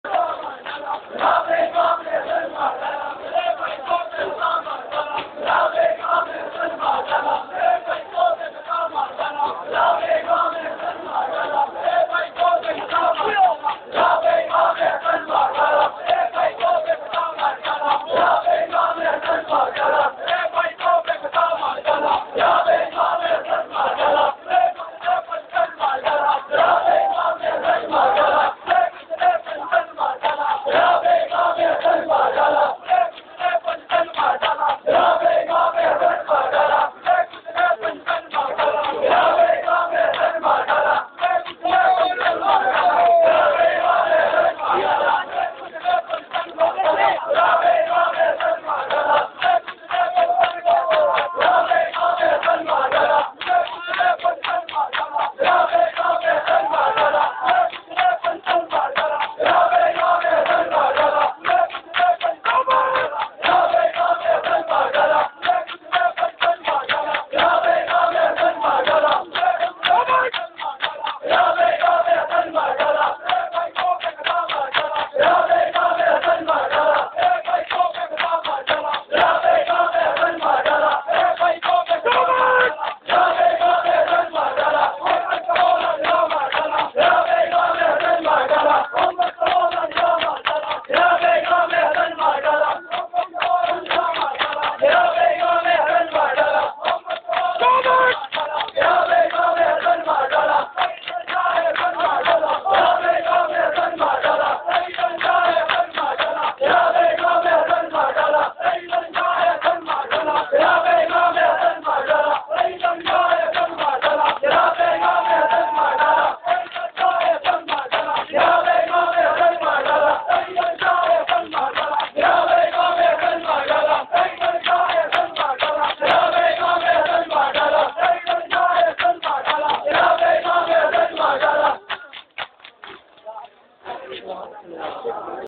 Ghayur 1
bawa GHAYUR  on 26th safar 2009 in markazi imam bargah islamabad